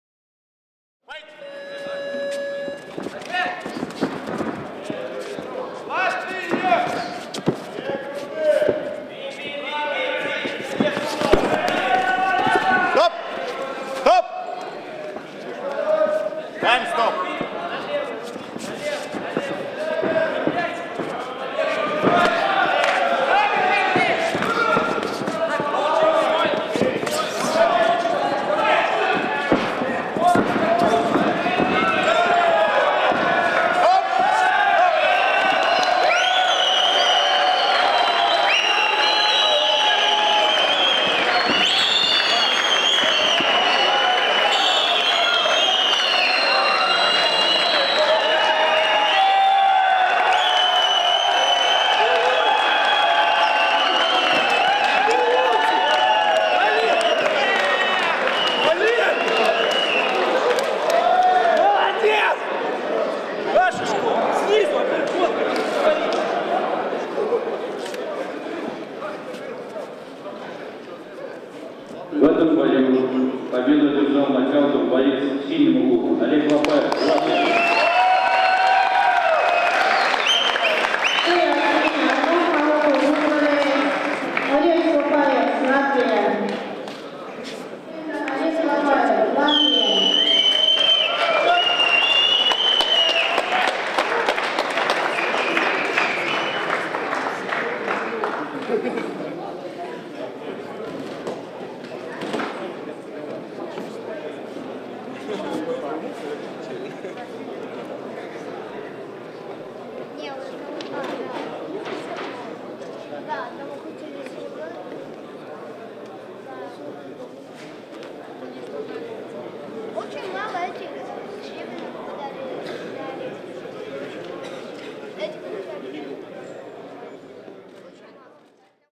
Sergey Trifonov (RUS) vs Olegs Lopajevs (LV)
EUROPEAN K-1 OPEN CUP 2012 RIGA/LATVIA финал, возрастная группа men, весовая категория - 91 кг.